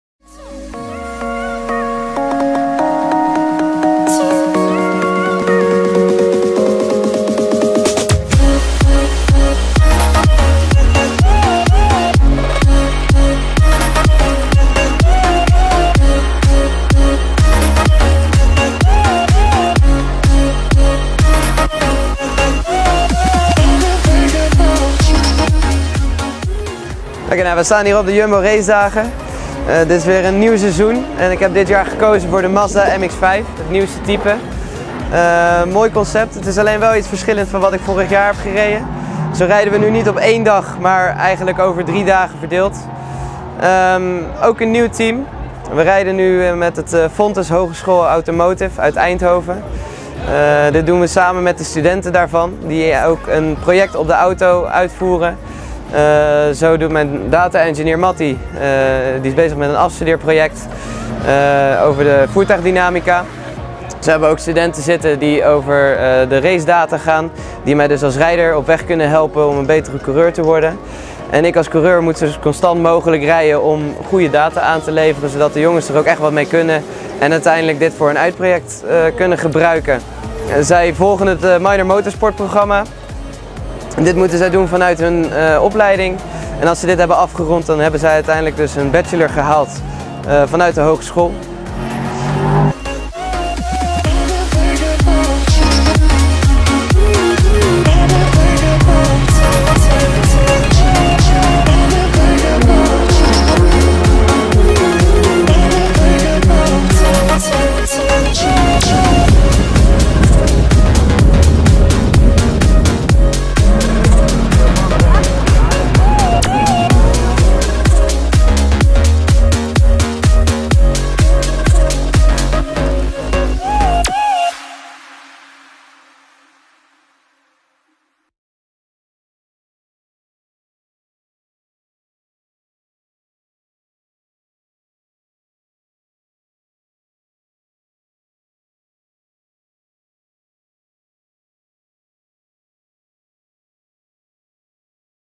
Fontys Minor Motorsport Engineering MX5 Cup Zandvoort 2019
Footage from our 2019 Zandvoort Cup race with Max de Bruijn